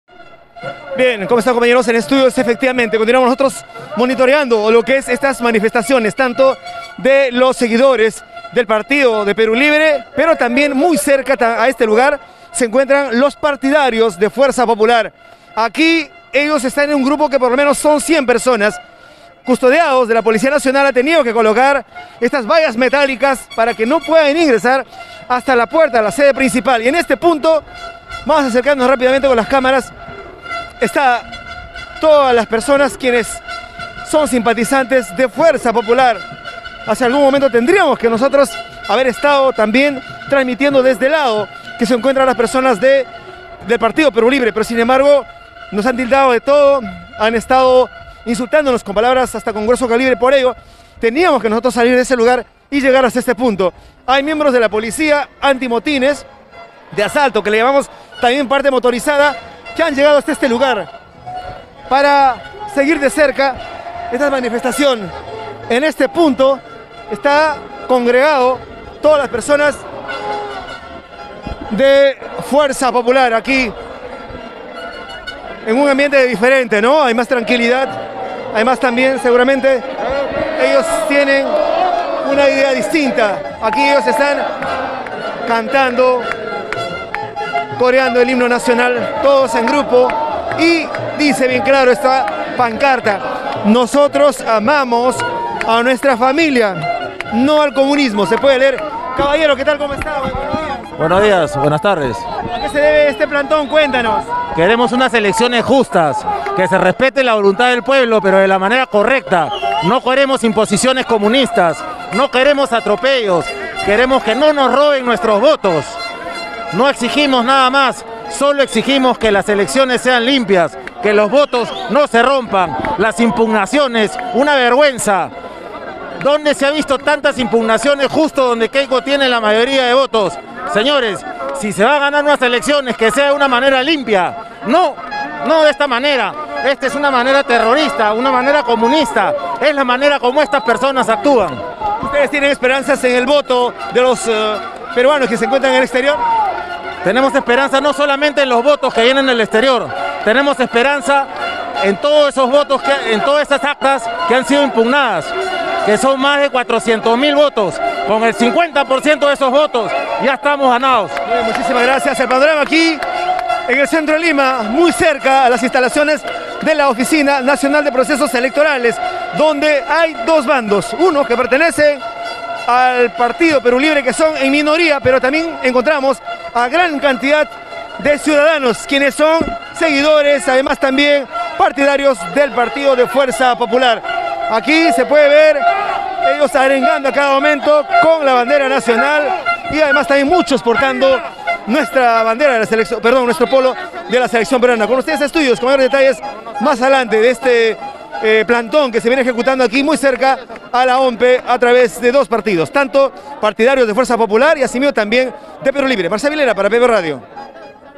PBO EN VIVO - MANIFESTANTES DE PERÚ LIBRE Y FUERZA POPULAR AFUERA DE LA ONPE
Elecciones2021 PBO ENVIVO 🗳️ KEIKO VS CASTILLO ¿FRAUDE ELECTORAL?

👉 Seguidores de Perú Libre y Fuerza Popular cerca de exteriores de la ONPE a espera de los resultados de la Segunda Vuelta

📻 91.9FM ¡LA RADIO CON FE!